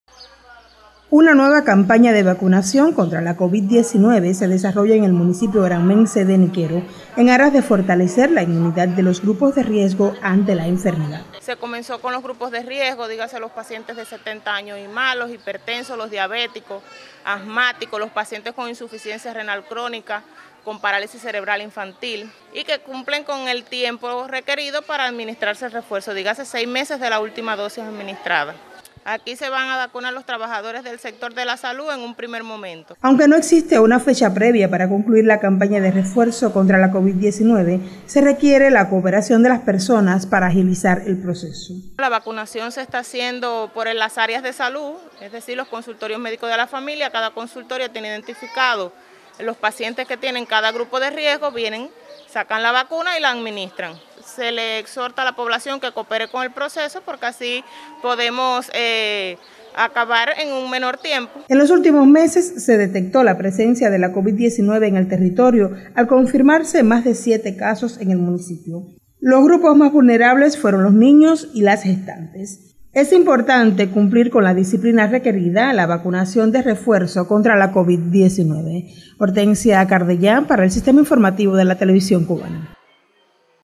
Avanza en Niquero campaña de vacunación de refuerzo contra la Covid 19
Personas mayores de 70 años y grupos de riesgo, entre ellos diabéticos, hipertensos y pacientes con otras enfermedades crónicas se incluyen en la nueva campaña de vacunación de refuerzo contra la COVID 19 en el municipio de Niquero. Un proceso requiere de la cooperación de la familia en aras de cumplir con el cronograma previsto en el sistema de atención primaria de salud de este territorio sureño de Granma.